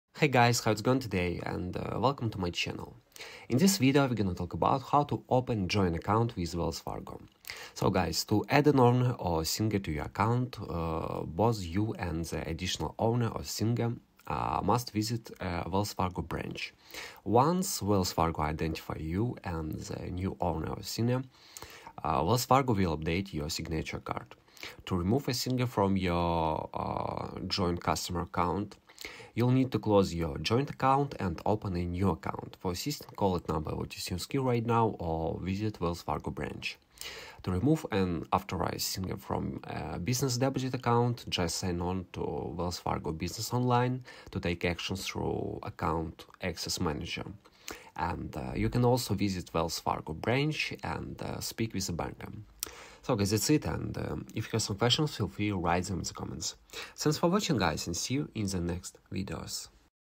Wells Fargo - how to open joint account
In this video we gonna talk about how to open joint account with Wells Fargo.